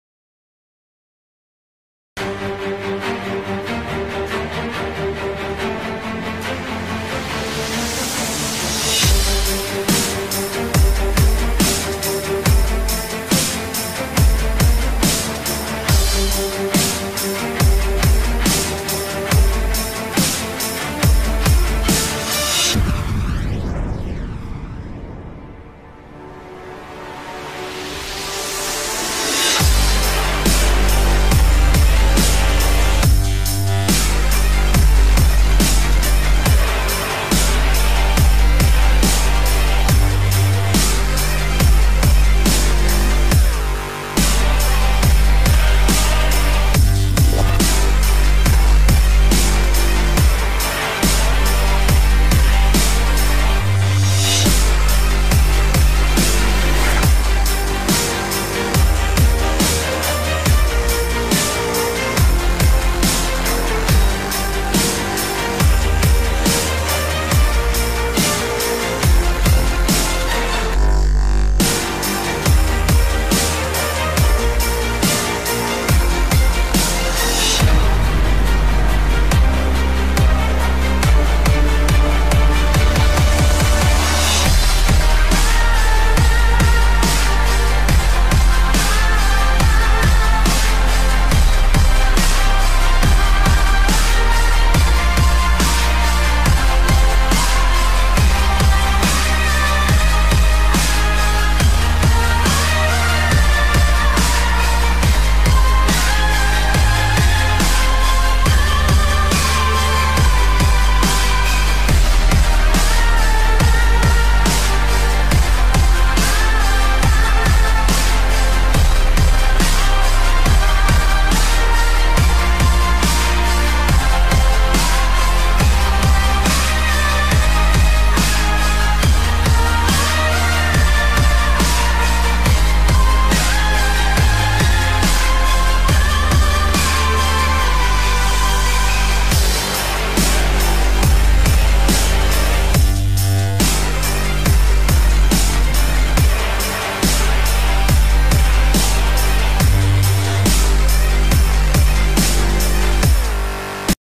Ukraine War - Russian army seize Ukrainian military base in Crimea Ukraine
With credits to Бандеренко
The present material selected, adapted & published by EMPR #1 independent citizen media about Ukraine | Russian Troops Seized Ukrainian Marines' Unit In Crimea, Mar 15 2014